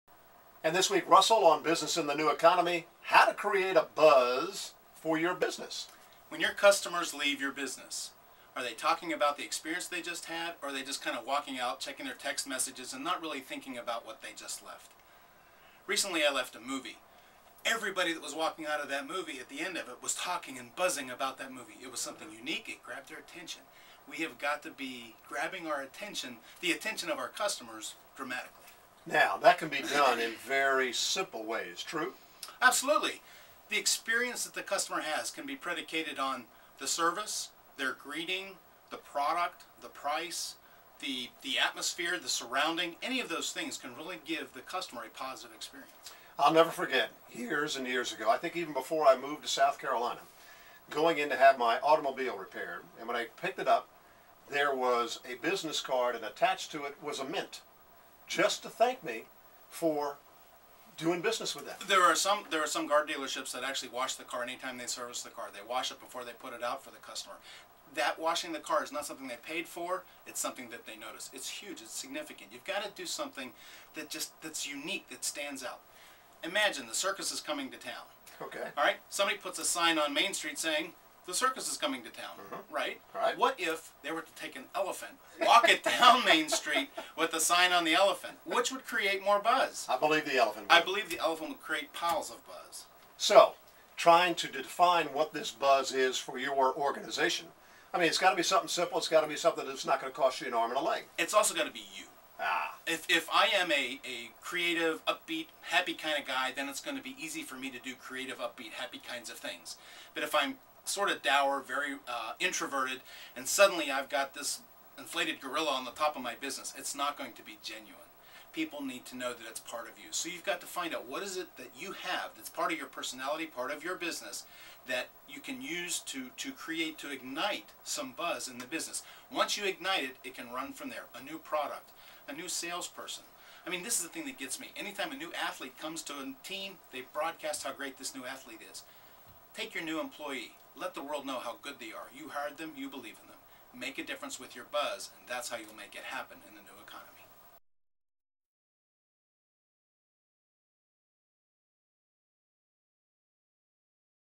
Create Buzz for Your Business
RussellWhite.com The key to creating buzz is based on either the experience you create for your customer, or a specific uniqueness that you build to stand out or having something specific to ignite the buzz. An event, product placement or something creative that gets positive awareness.